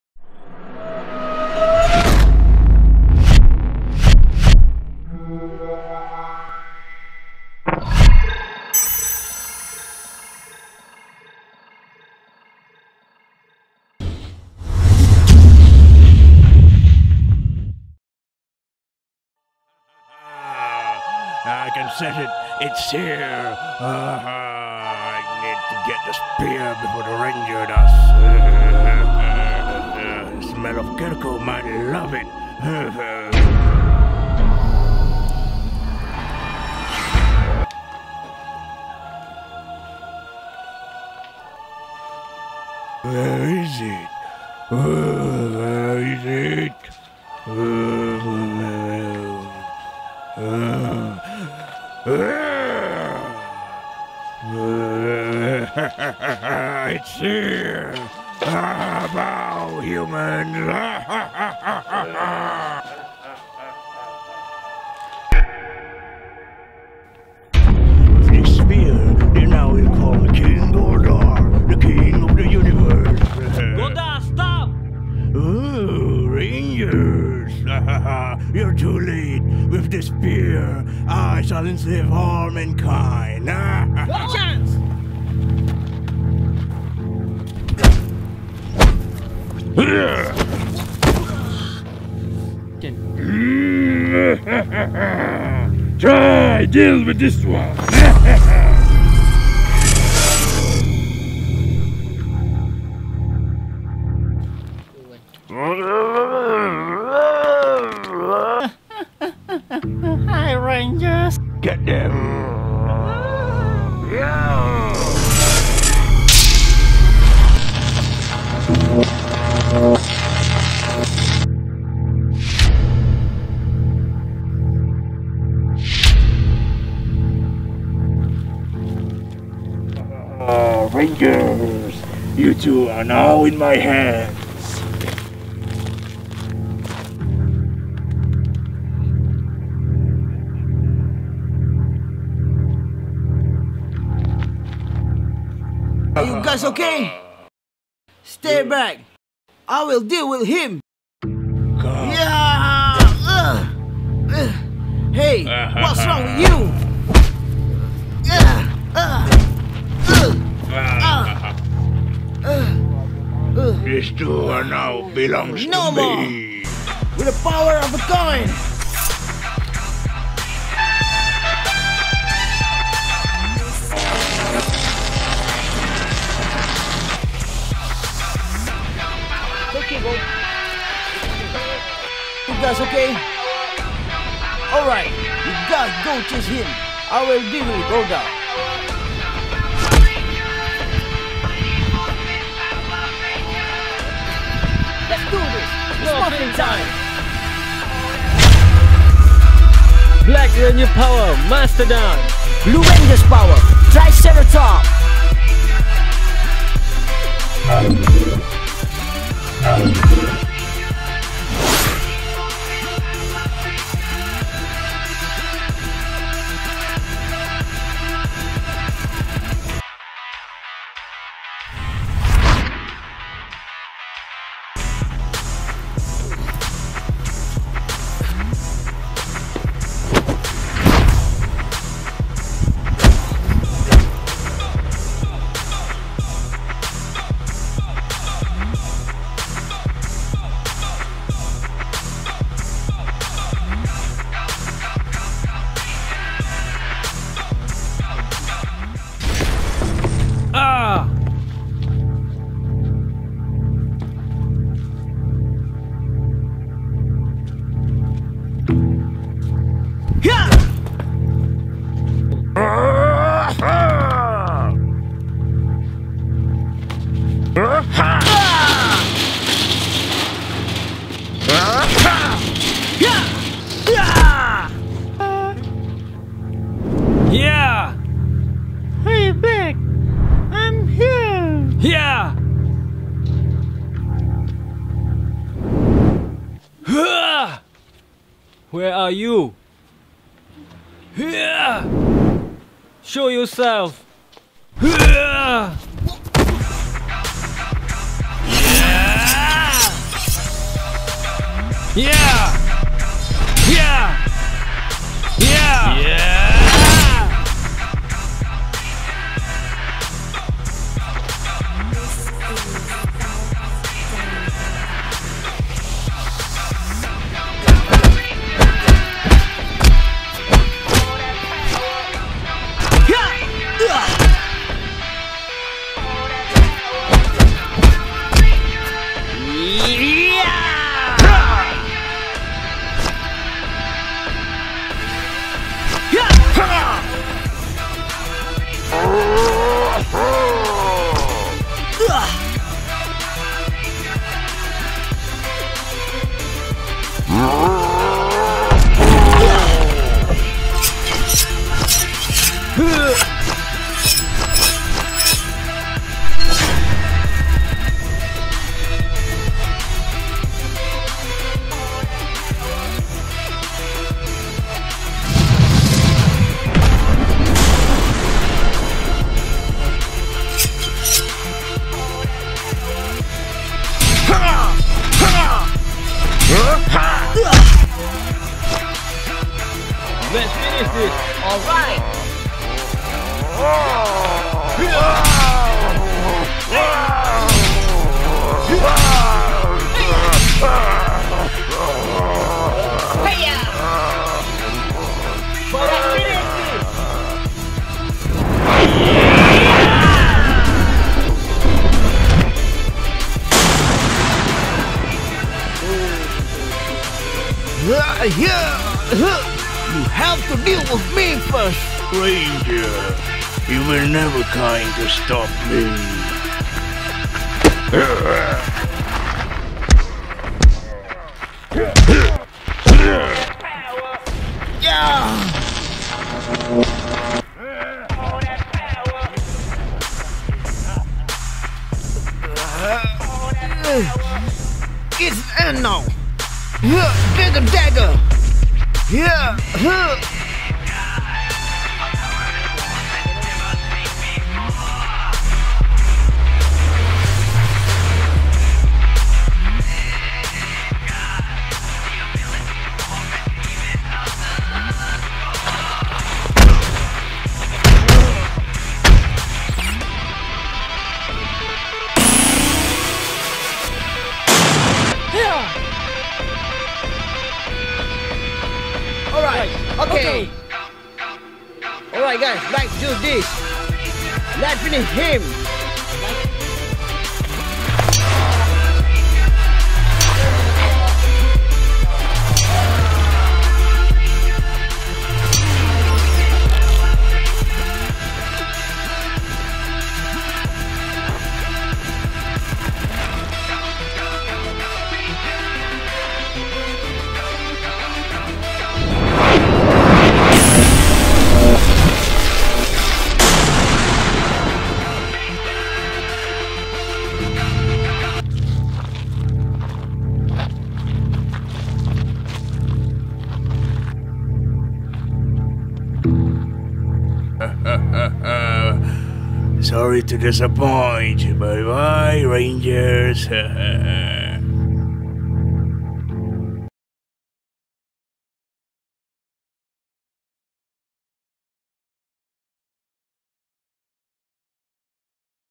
POWER RANGERS ON THE RUN ! - Power Rangers Fan short Film
Brotherhood of Heroes are in collaboration with Field studio in making our short fan film
Of Power Rangers/ Super Sentai.

Every prop except for the spear head Goldar use are made by Brotherhood of Heroes. The spear is a custom ultrasaber light saber.

Suits actors and actors are members of Brotherhood of Heroes.

Field Studio did an amazing job edditing and co direct.

We would like to thank everyone for making this possible and thank you for your support.